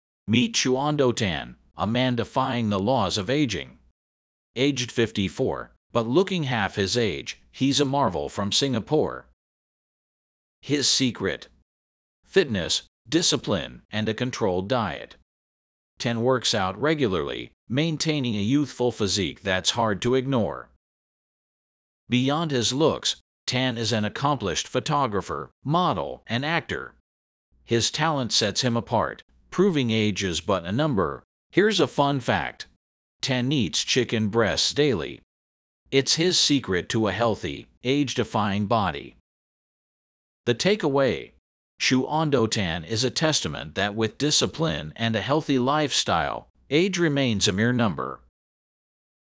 Chuando Tan doesn’t grow old
He became famous overnight.
He looks 40 years younger than his usual age.
ChuandoTan is a man who doesn’t grow old.